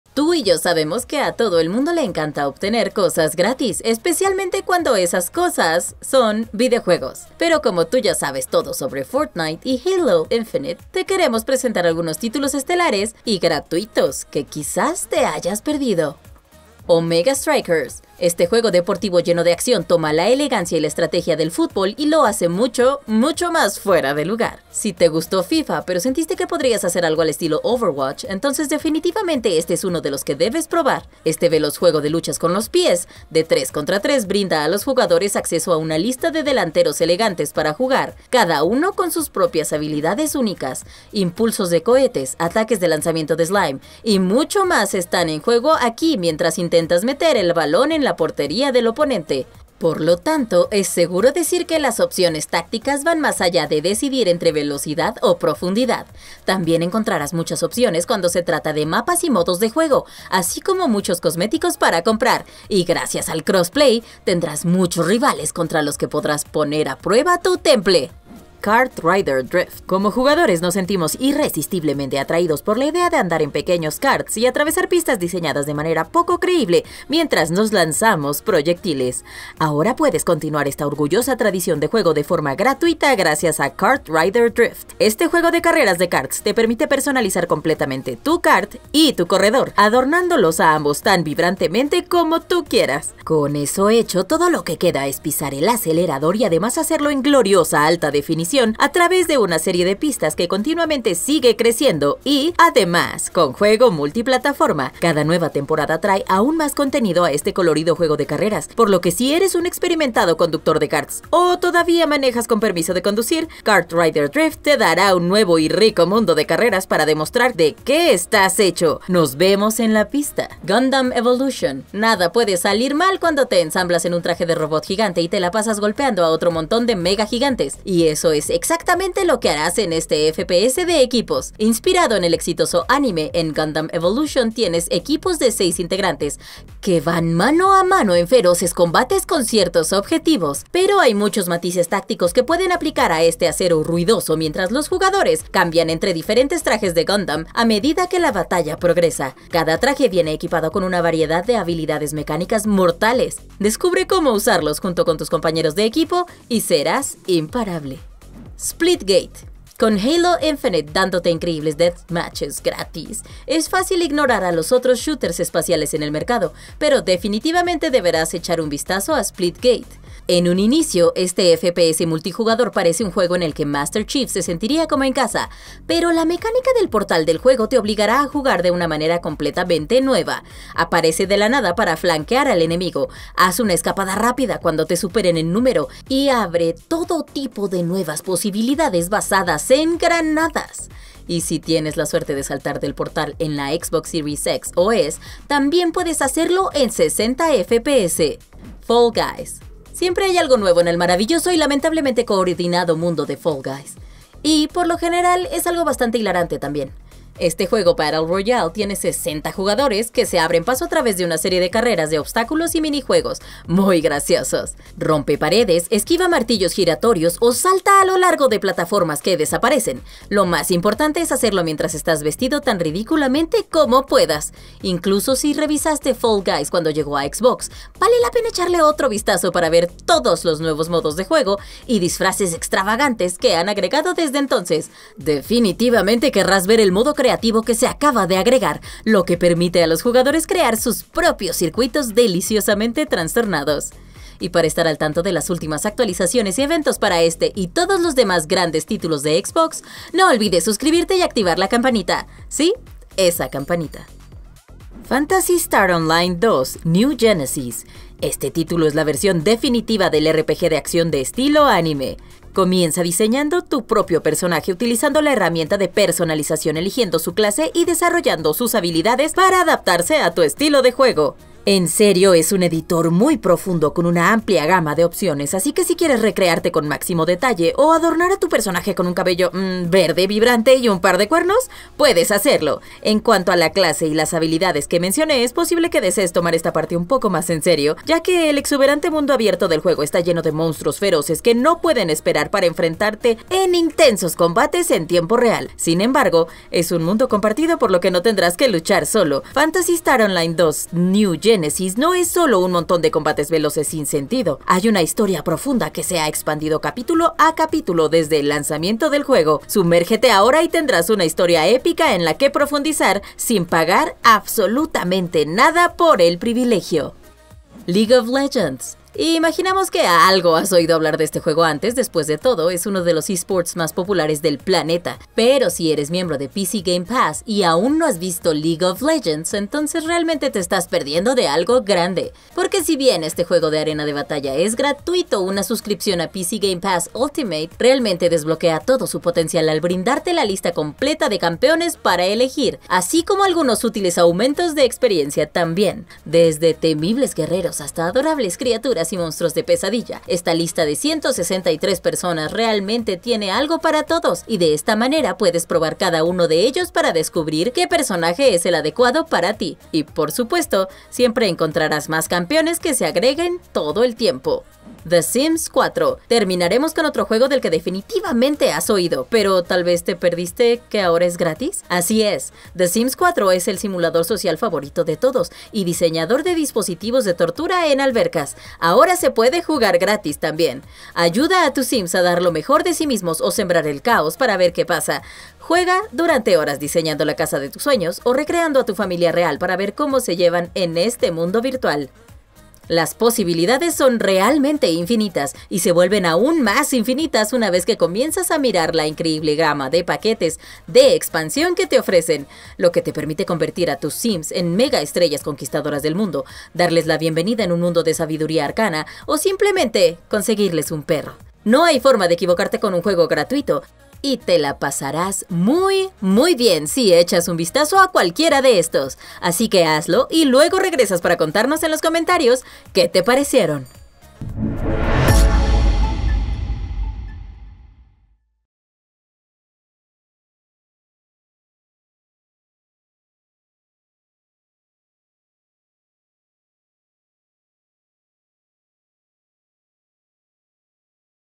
Los mejores juegos GRATIS para tu Xbox Series y PC de este 2023
Si estás buscando los mejores juegos gratuitos de 2023, llegaste al lugar indicado – desde juegos de futbol un poco diferentes, hasta multijugadores MASIVOS. Los jugadores de Xbox y PC tienen muchas opciones que probablemente pasaron por alto. Aquí te dejamos lo mejor de lo mejor en juegos gratuitos de este 2023 para Xbox y PC.